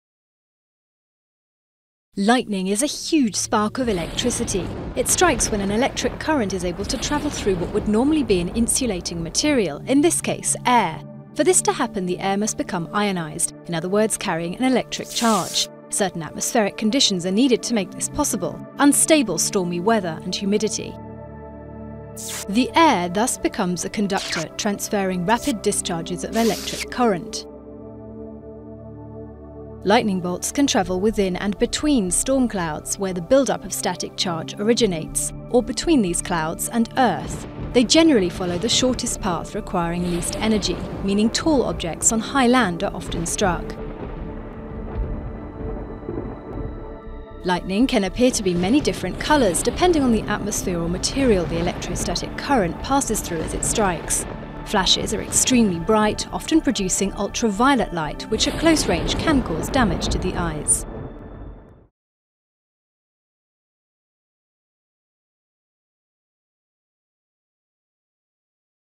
Lightning
Bangladesh has begun planting one million palm trees nationwide to help prevent hundreds of people being killed by lightning strikes every year. Authorities last year declared lightning a natural disaster as official tallies recorded more than 200 deaths in 2016, with 82 people dying on a single day in May.VIDEOGRAPHIC